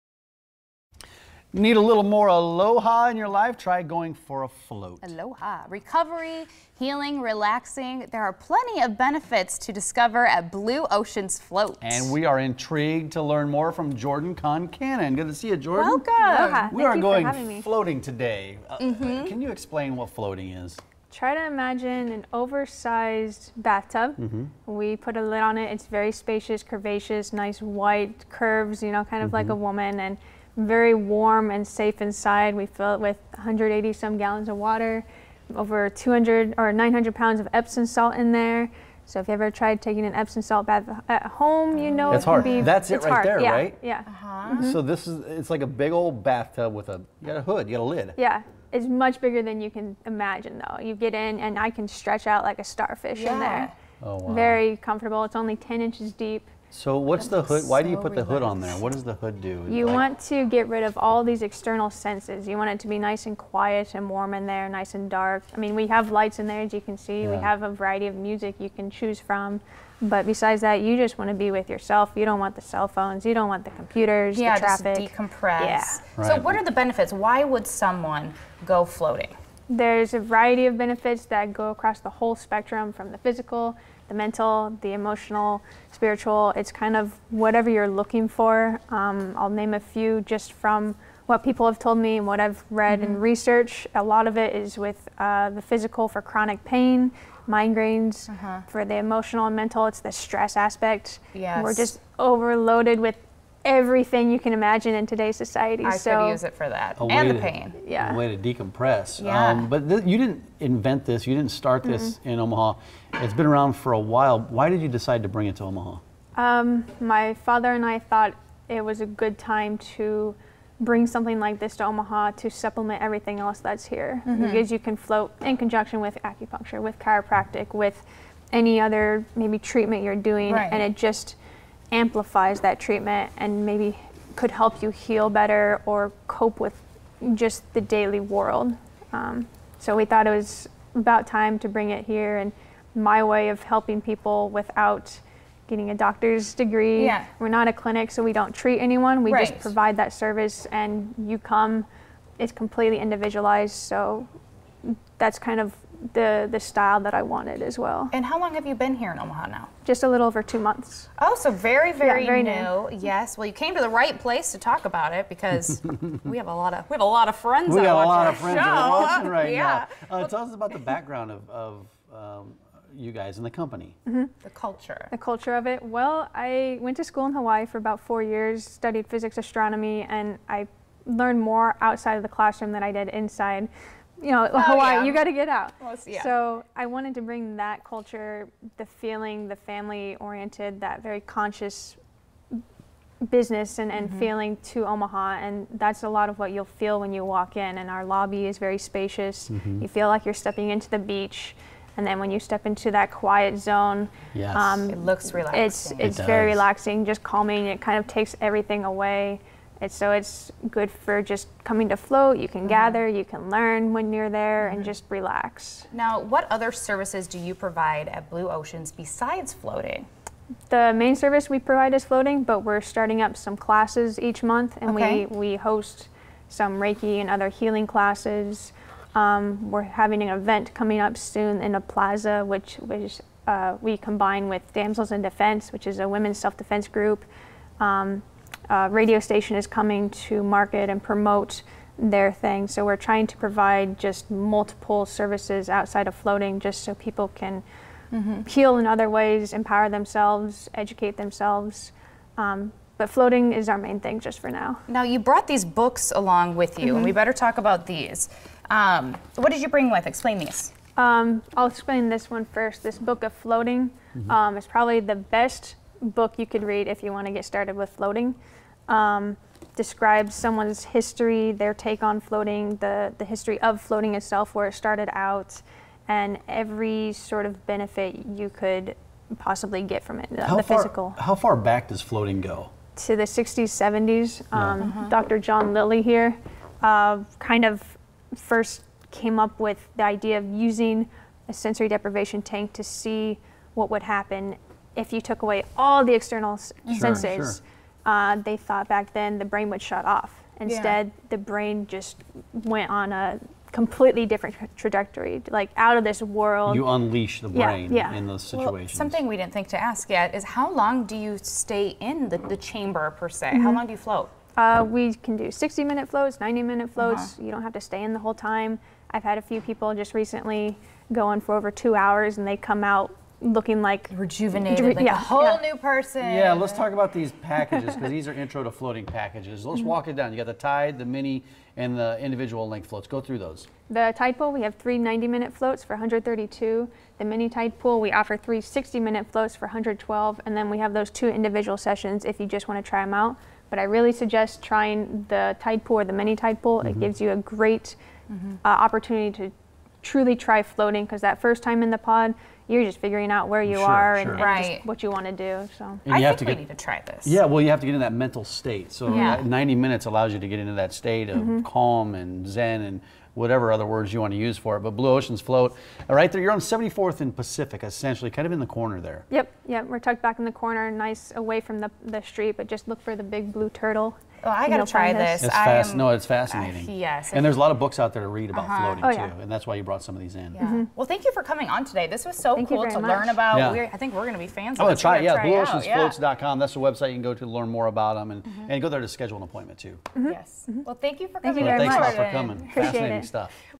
Blue Oceans Float
Blue Oceans Float provides floating for Omaha and today we're intrigued to learn more.